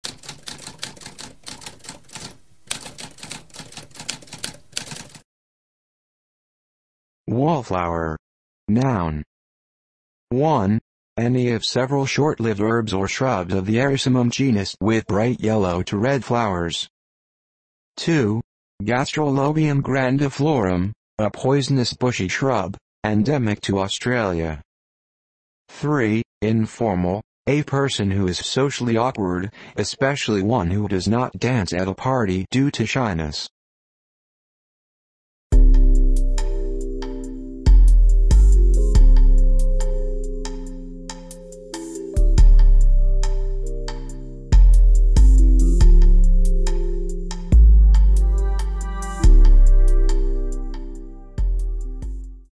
What does wallflower mean?
A spoken definition of wallflower.

Intro Sound:
Typewriter - Tamskp